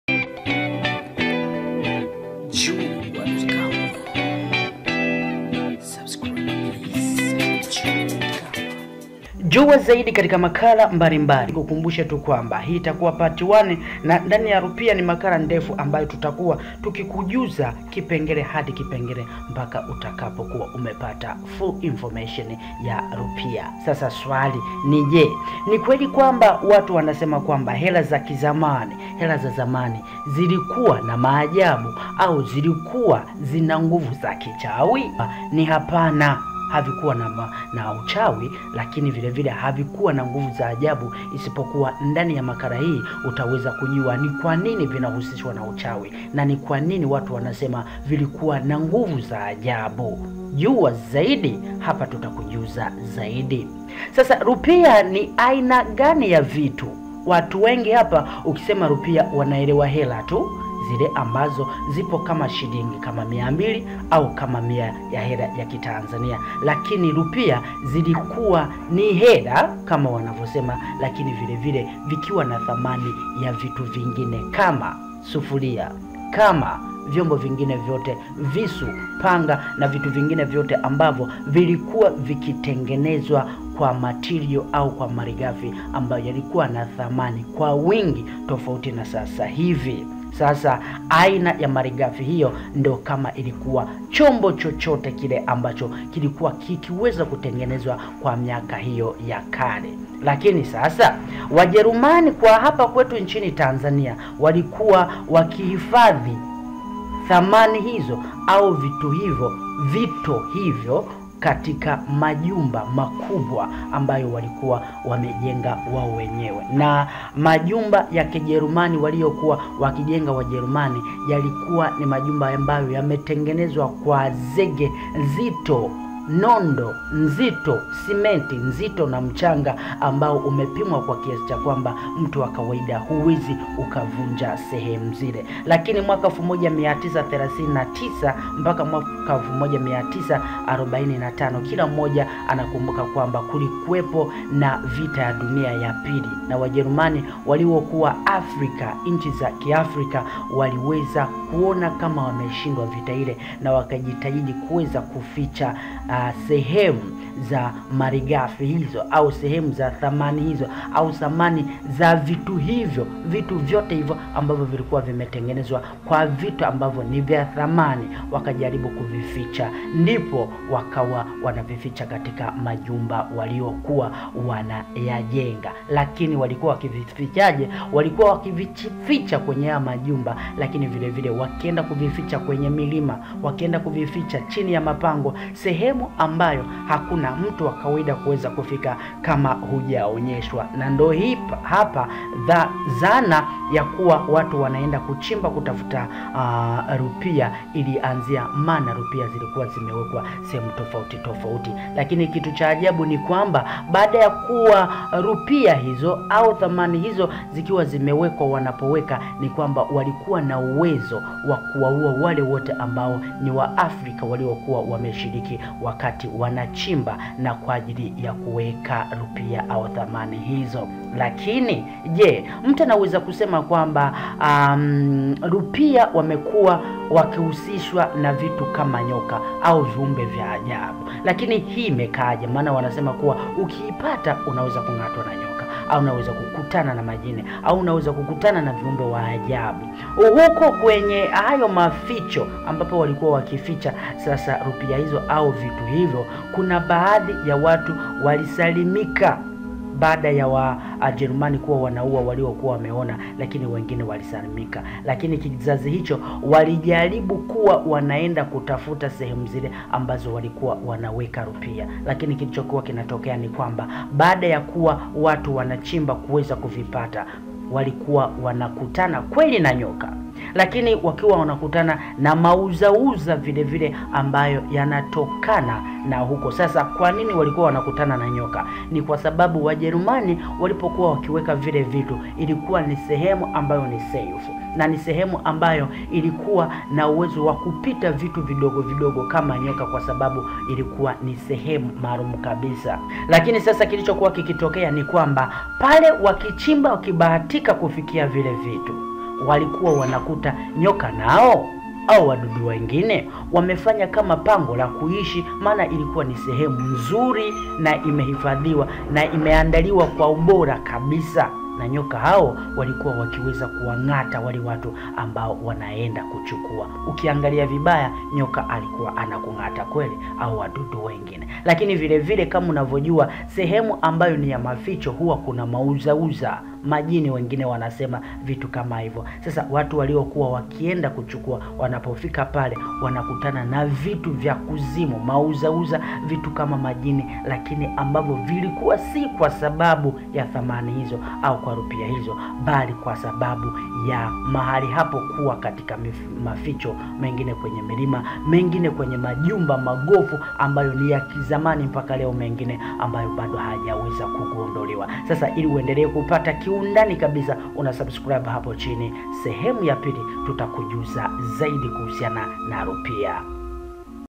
HII NDIO RUPIA MALI KUBWA DUNIANI
Rupia ilikua mali kubwa sna na sasa bado inasakwa kwa hali na mali